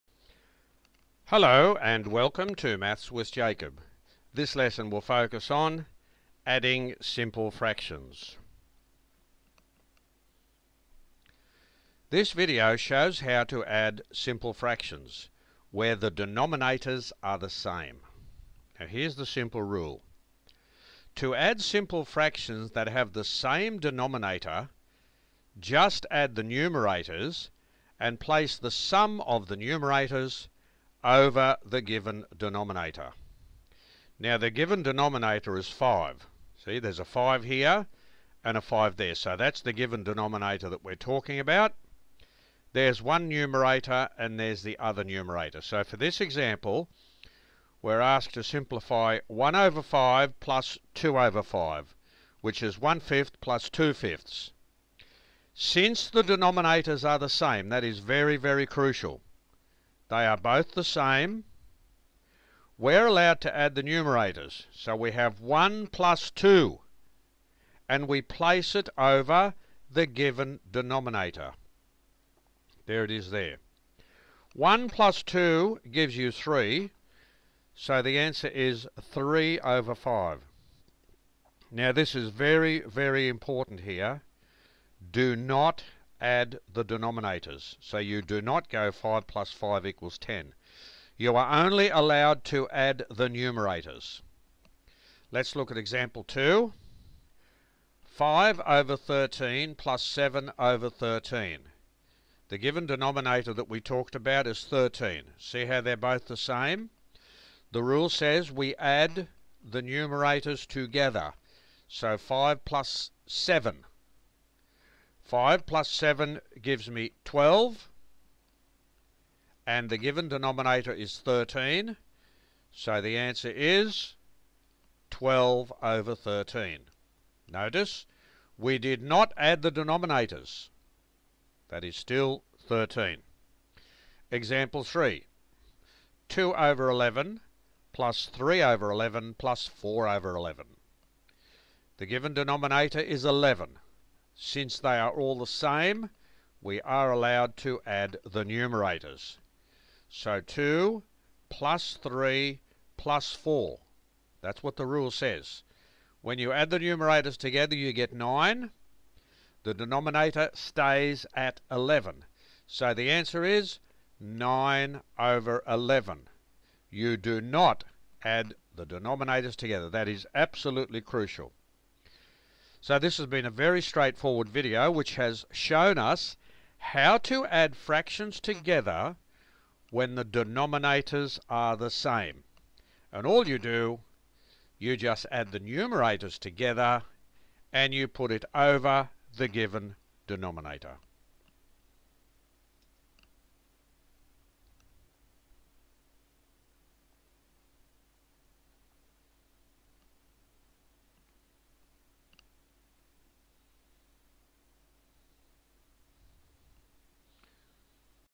Adding Simple Fractions
this video gives us a simple strategy for adding simple fractions.